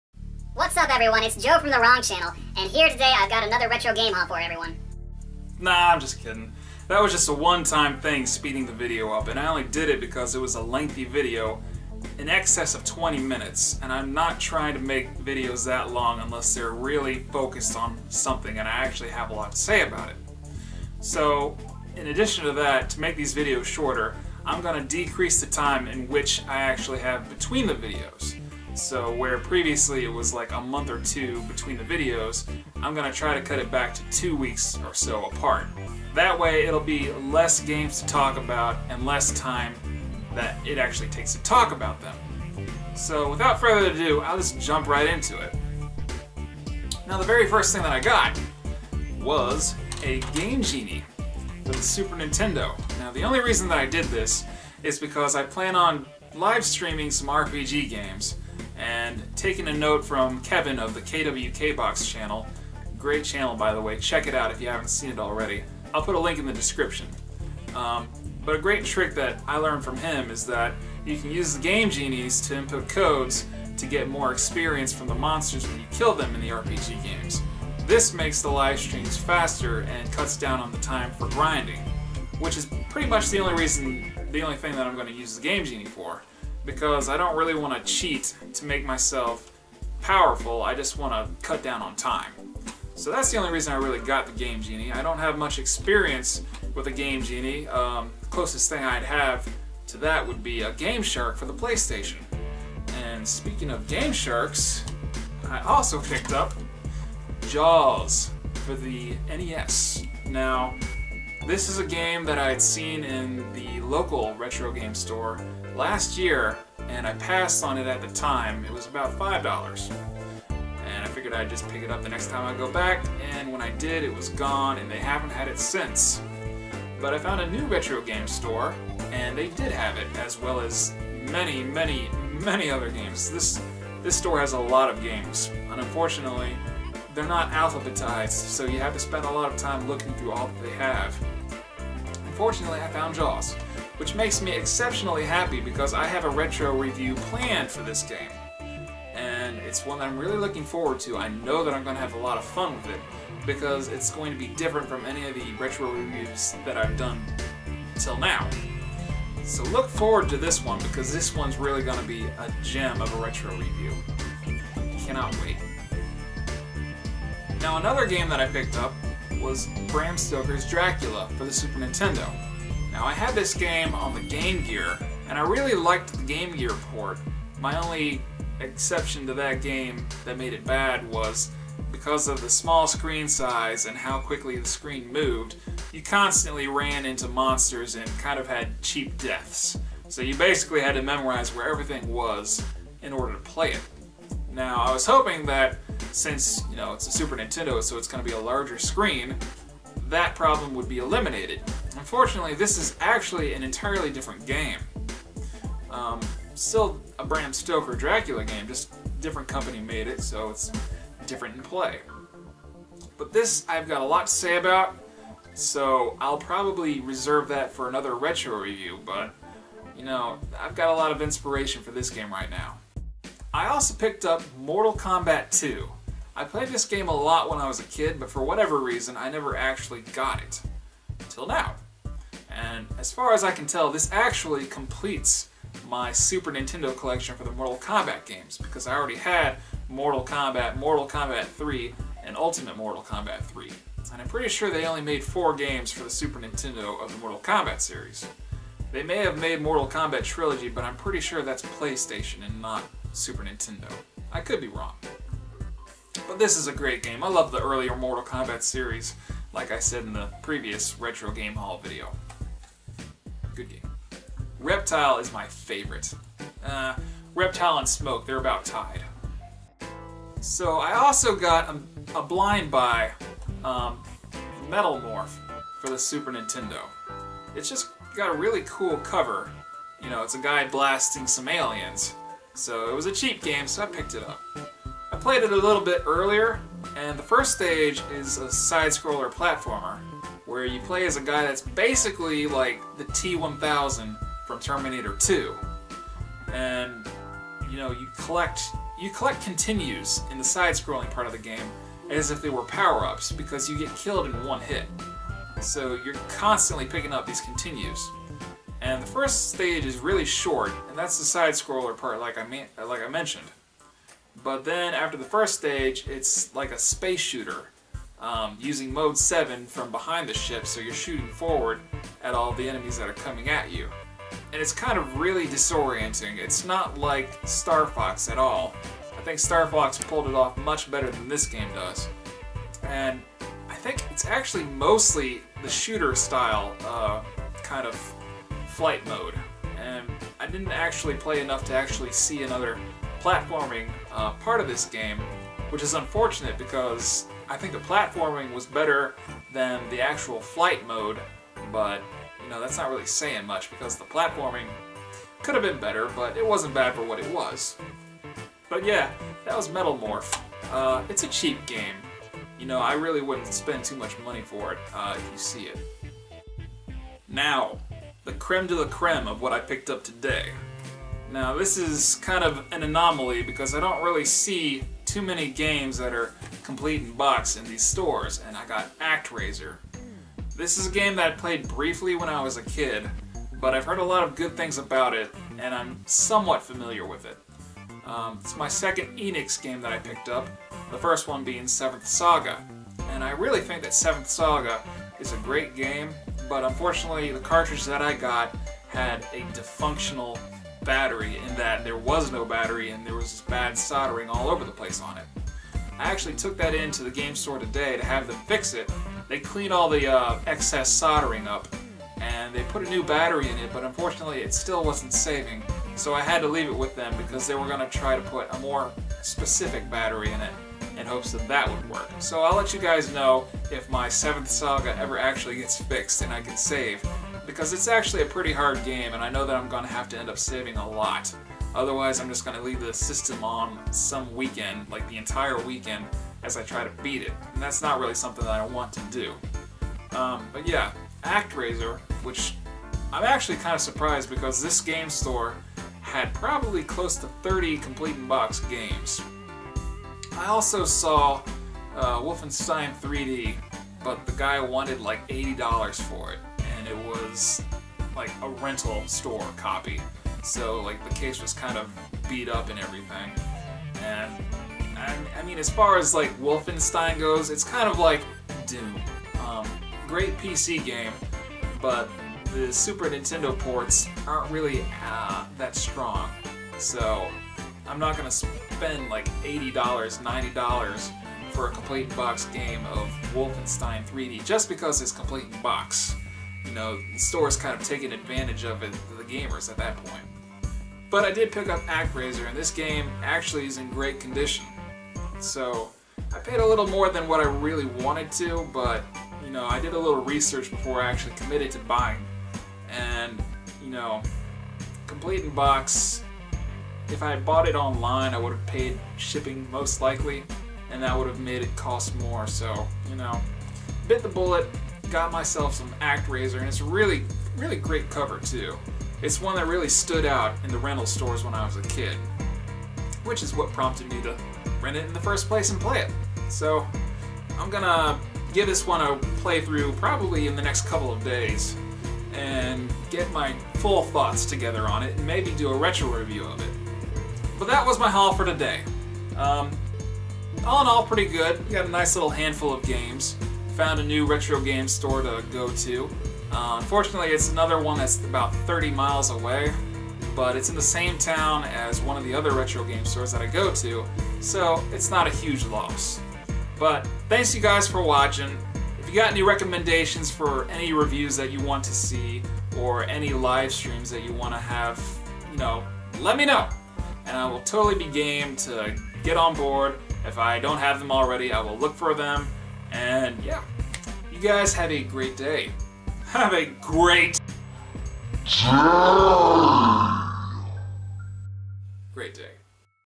Retro Game Haul 6
I'm at it again! I've got the retro game fever, and no cowbell! In this video, I talk about a new game store I found as well as some new goodies. Enjoy!

ALSO! Check out KWKBOX's channel. He does some great retro gaming videos from game nights to live streams!